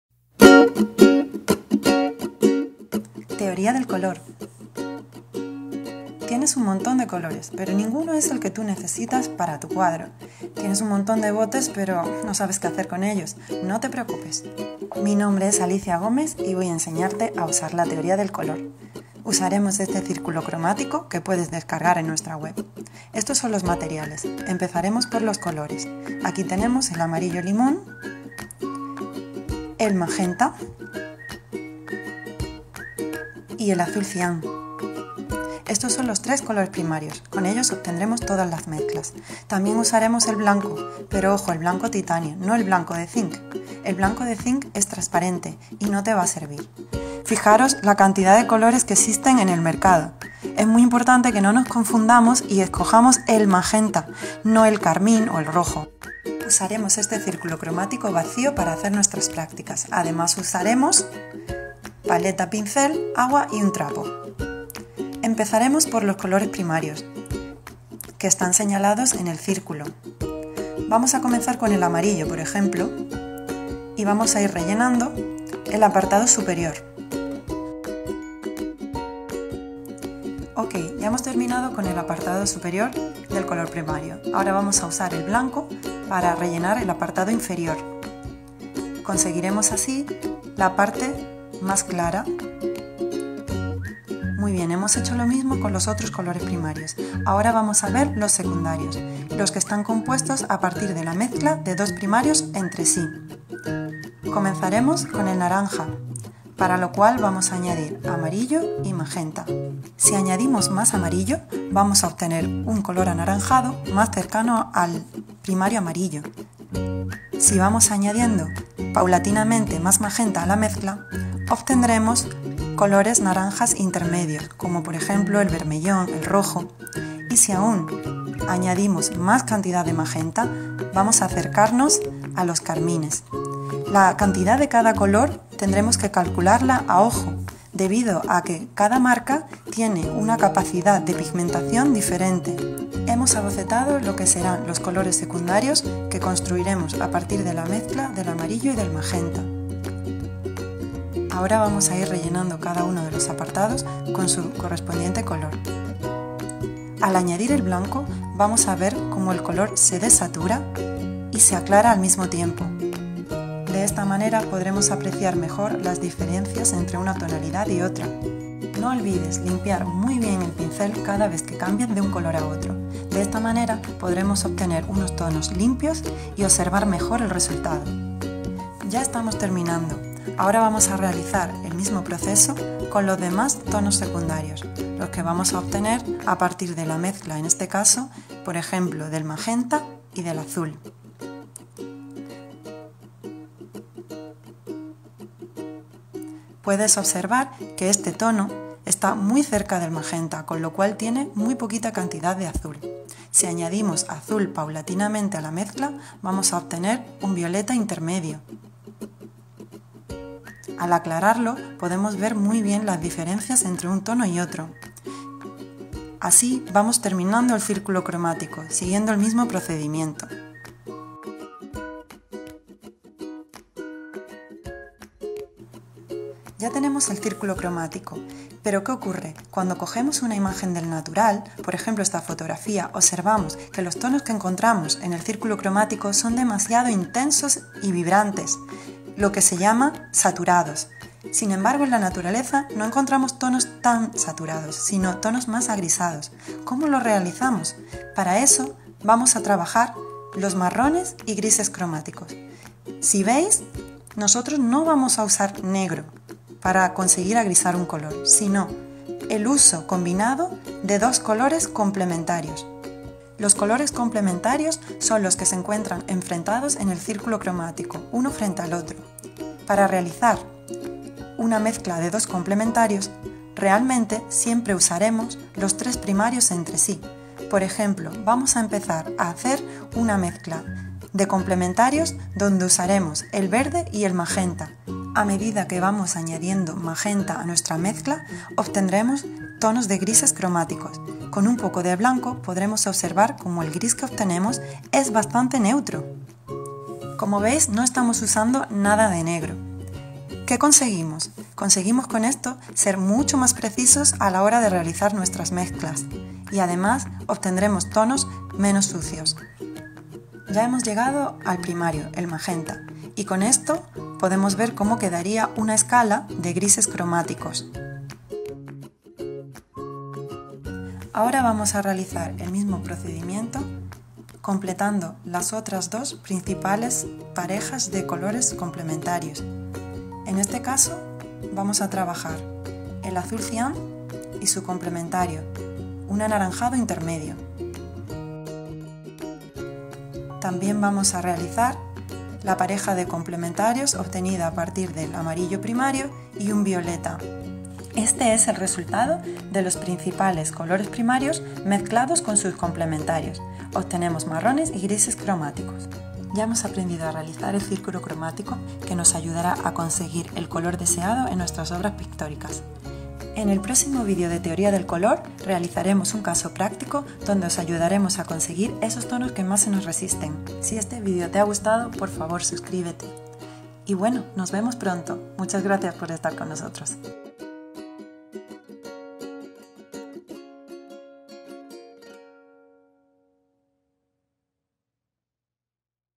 teoria del color I. Cómo mezclar colores básicos
Con el círculo cromático aprenderás la teoría básica del color para conseguir cualquier color a partir de los 3 colores primarios.

Profesora: Alicia Gomar, licenciada en Bellas Artes, directora de ARTÍSTICA, Academia de Bellas Artes desde su fundación en 2012.

Dudas respondidas en los comentarios:
"Creía que los primarios eran el amarillo, rojo y azul"
RESPUESTA: los primarios son los tres únicos colores con los que podemos crear todos los demás excepto el blanco. Las impresoras, por ejemplo, tienen esos tres colores en sus cartuchos Cian, Amarillo, Magenta y además el negro,  (para imprimir sólo en blanco y negro) (CMYK, las siglas en Inglés). Si una impresora puede sacar casi todos los colores nosotros también, el problema es que muchas marcas comerciales venden el rojo o el azul ultramar con el nombre de primarios, debido a una gran ignorancia. Ejemplo: el rojo contiene amarillo y magenta, por lo cual es un color secundario y no nos permitirá crear colores limpios como el violeta, puedes intentarlo en casa, si mezclas el rojo y el azul obtendrás un violeta sucio).

"Porqué no aclaras los porcentajes de cada color para realizar las mezclas"
RESPUESTA: Respecto a los porcentajes que solicitas, no podemos especificarlos porque esto va a depender de la pureza del pigmento de cada color y esto varía según la marca usada. Por ejemplo, en la marca Mir el magenta tiene más poder de pigmentación que el cian, por lo que para conseguir un violeta intermedio la cantidad de magenta debe ser inferior que la del cian, pero sin embargo en la marca Sennelier, el cian es super potente y con sólo añadir una pizca de cian y casi todo de magenta tendrás el mismo color. Además, no es lo mismo si se usa acrílico, óleo o cualquier otro material. La verdad es que en el área de la pintura no se puede ser muy matemático puesto que hay muchos factores que influyen y al final lo que realmente importa es la creatividad y la capacidad de experimentación de cada artista.

"¿Porqué usas un círculo cromático?"
RESPUESTA: El círculo es una buena herramienta porque enfrenta diametralmente a los colores primarios con sus complementarios. También sirve para armonizar y comprender los análogos, aunque eso es material que debería ser tratado en otro vídeo.